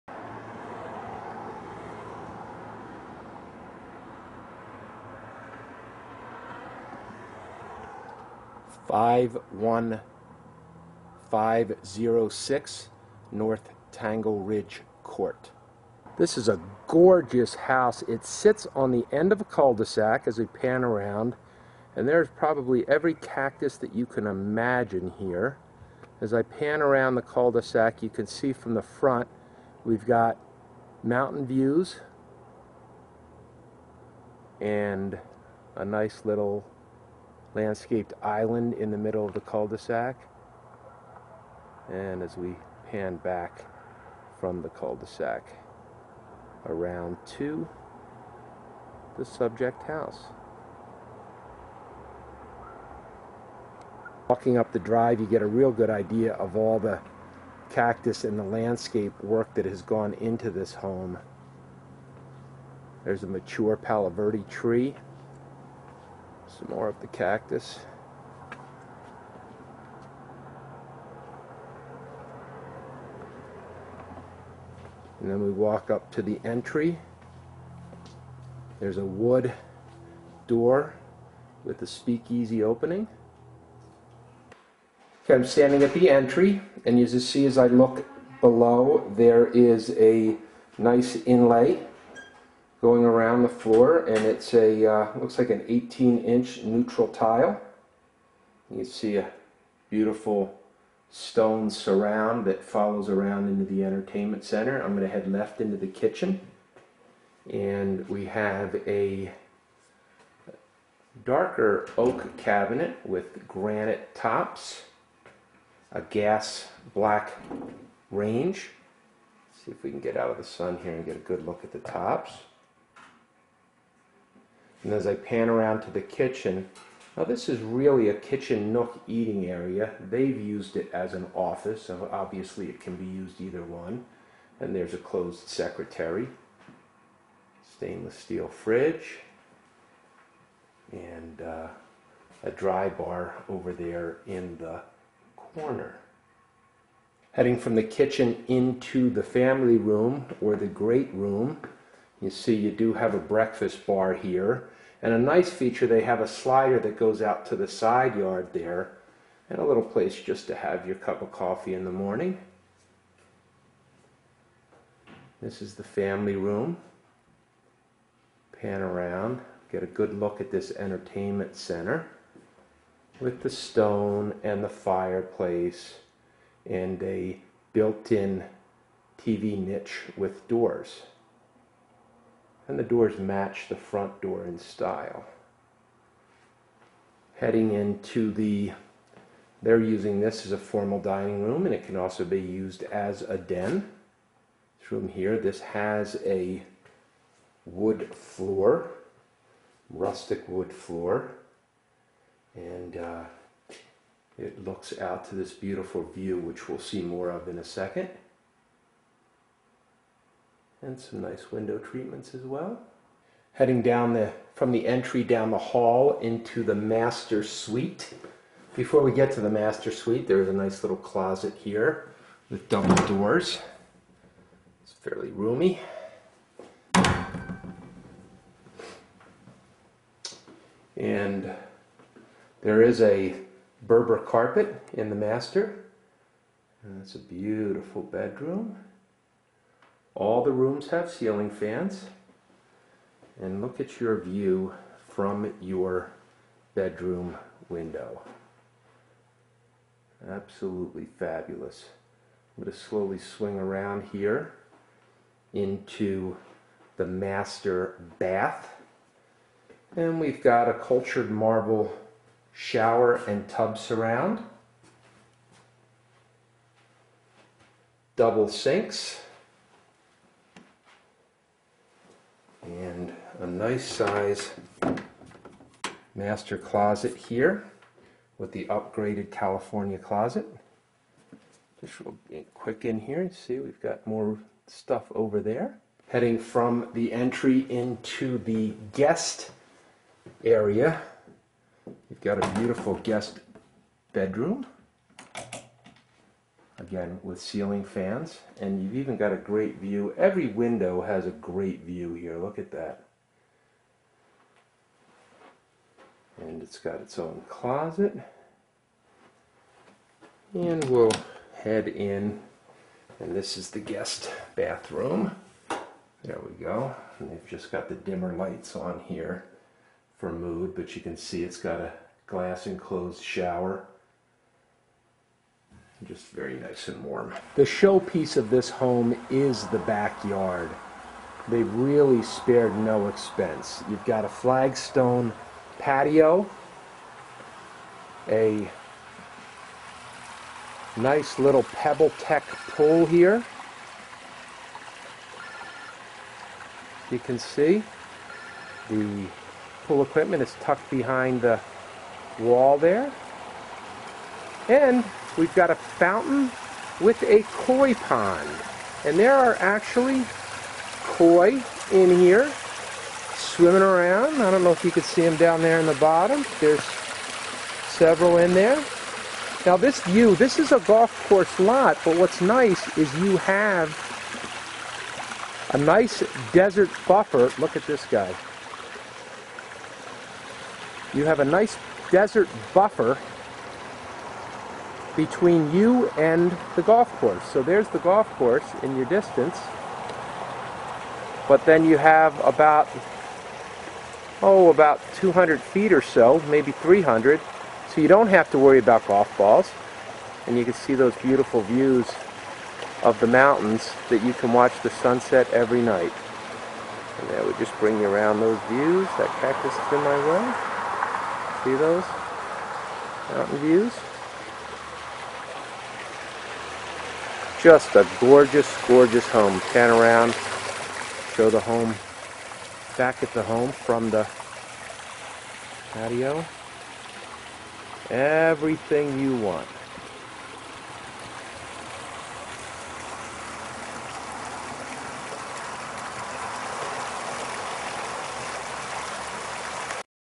41506 N Tangle Ridge CT Anthem AZ
This is the one! Model perfect home with resort style yard and an awesome view! Prime location overlooking natural arroyo and the tenth green of Persimmon Golf course. Custom brick/stone fireplace and built in entertainment niche. Enjoy the sunset in your incredible backyard featuring a custom pebble-tec pool, flagstone patio, and walkway to a koy pond with a rock waterfall The kitchen features upgraded appliances and solid granite counters. Come see it and fall in love!